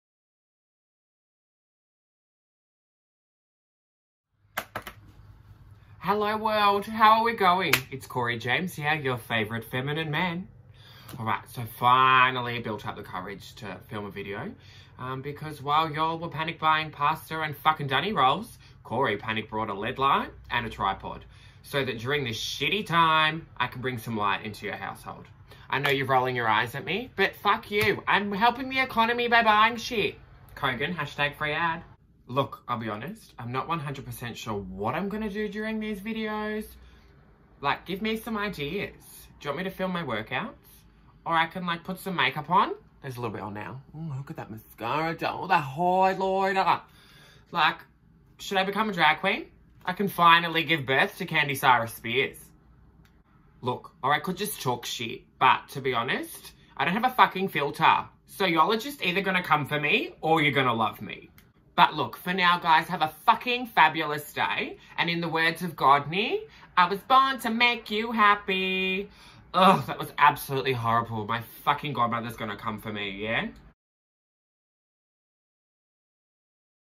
HELLO WORLD
Sorry for the swear words. Hello I just wanted to introduce myself and make you laugh during this pandemic xx